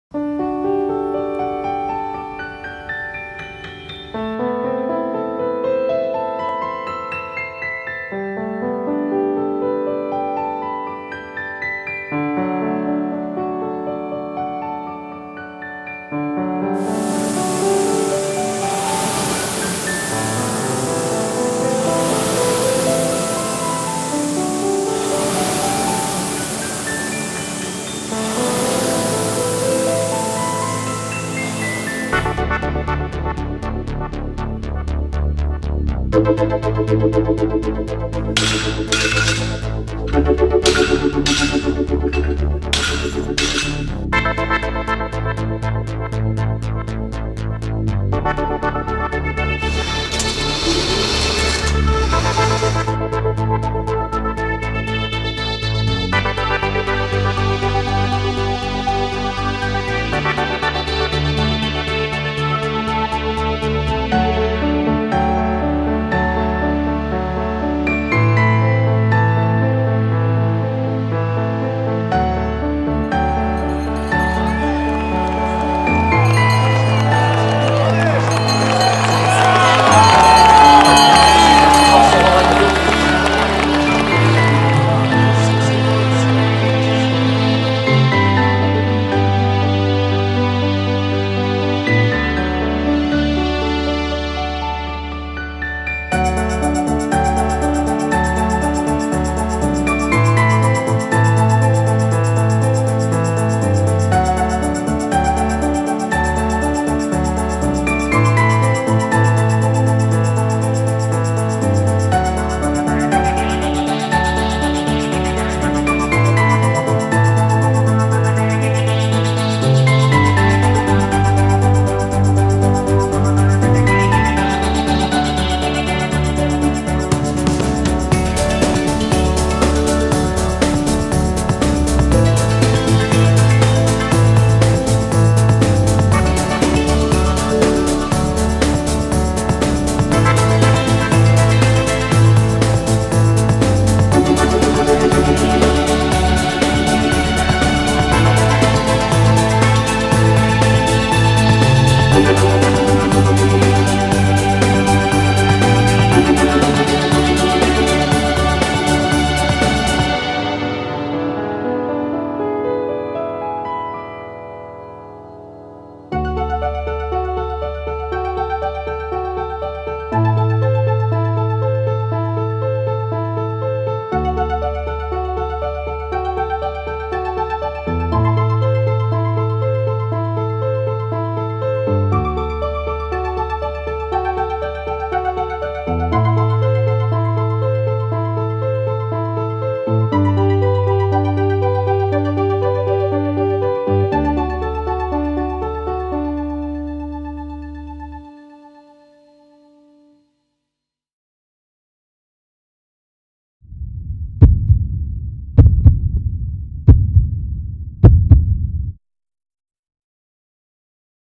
Olympia 2012 London Gold Medal mens eight
Olympia mens eight Eric Johannesen Rowing Club Bergedorf
Eric siegt 2012 im Männerachter